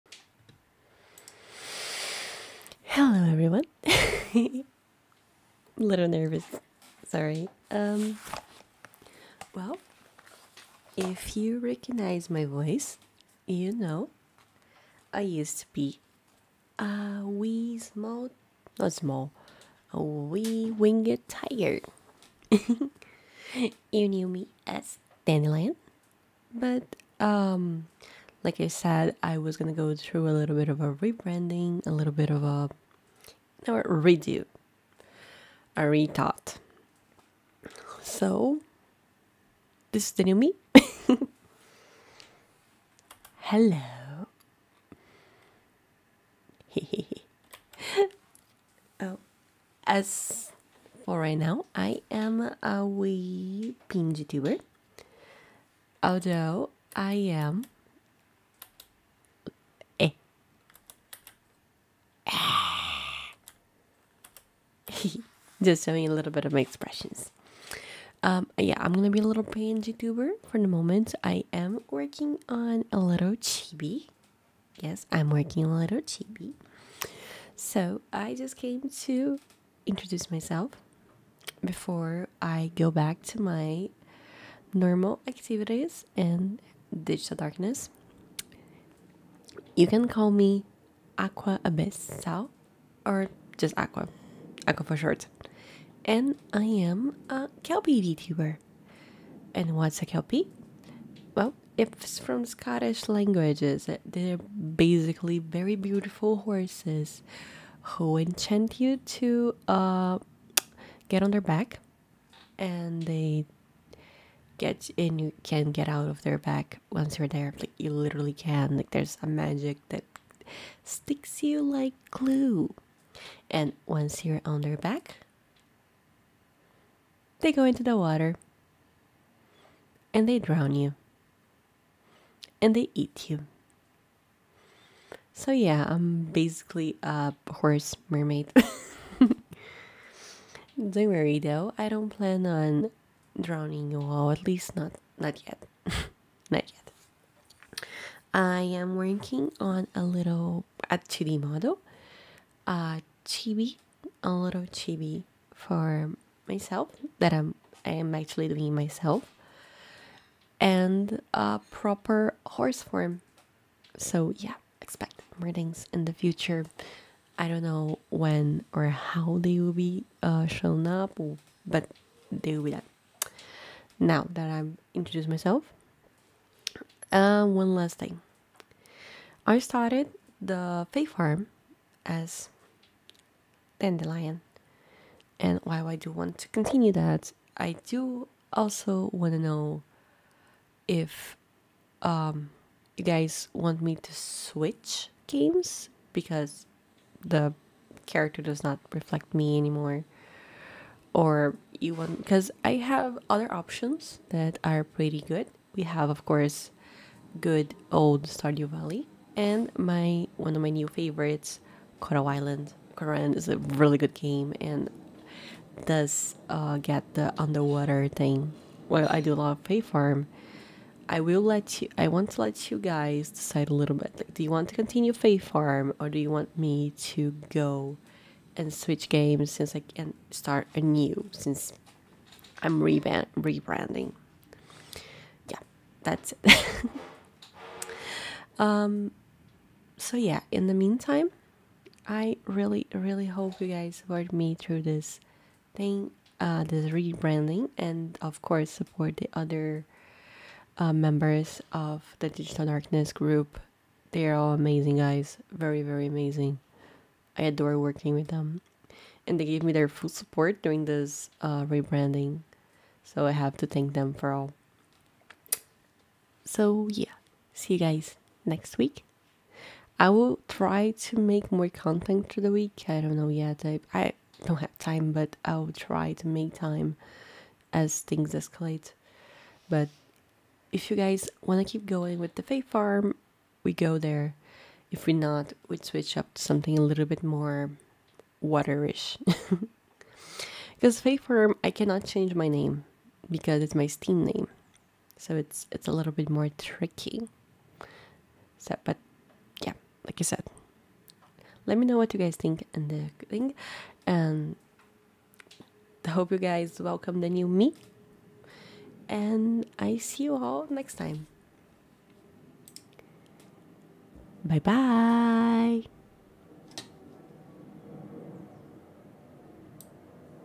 Dandelion's rebranding!
Hello everyone! I'm announcing my return with my new name and design, and  making a little question for you all:
Would you like to continue with Fae Farm or shall we switch to a game more within the vibe of the vtuber?